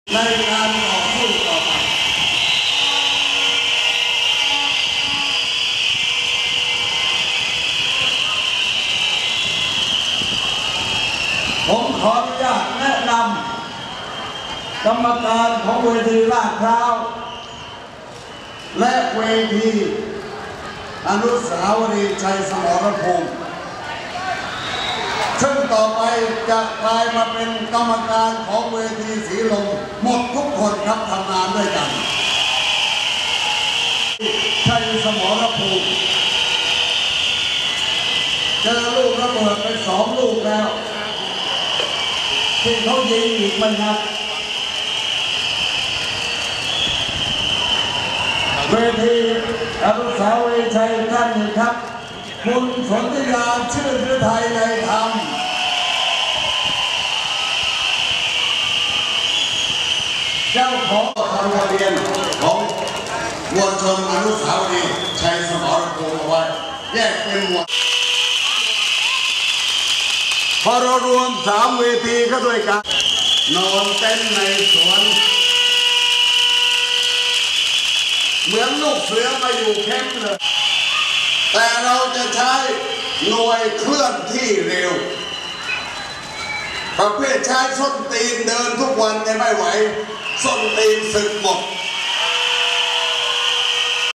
ม็อบกปปส.เวทีสวนลุม
เลขาธิการ กลุ่ม กปปส. นายสุเทพ เทือกสุบรรณ เดินทางมาขึ้นเวทีสวนลุม เพื่อแนะนำแกนนำ จากเวทีอโศก และราชประสงค์ ซึ่งจะยุบรวมมาตั้งที่สวนลุมเพียงที่เดียว เพื่อความปลอดภัยของมวลมหาชนที่มาร่วมชุมนุมขับไล่ รัฐบาลรักษาการของ น.ส.ยิ่งลักษณ์ ชินวัตร Photographerชาญณรงค์ พรดิลกรัตน์ (Charnnarong Porndilokrad)